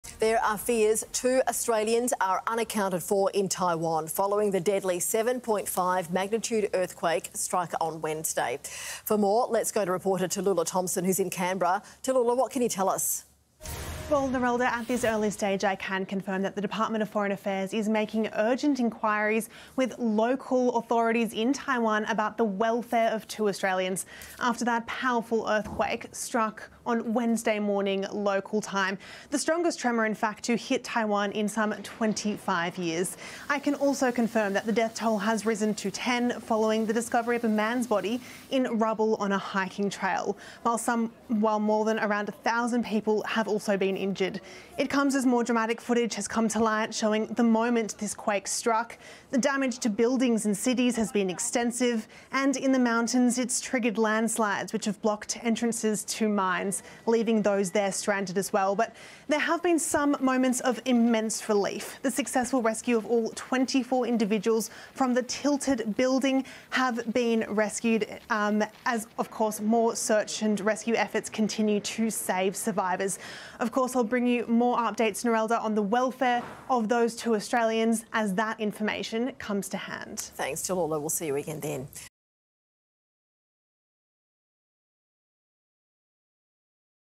Two Australians Feared Missing After Taiwan Earthquake | 10 News First
There are fears two Australians are missing in Taiwan, following the deadly earthquake that struck on Wednesday.

So far, 10 people have been killed, with over a thousand injured, according to the national disaster agency.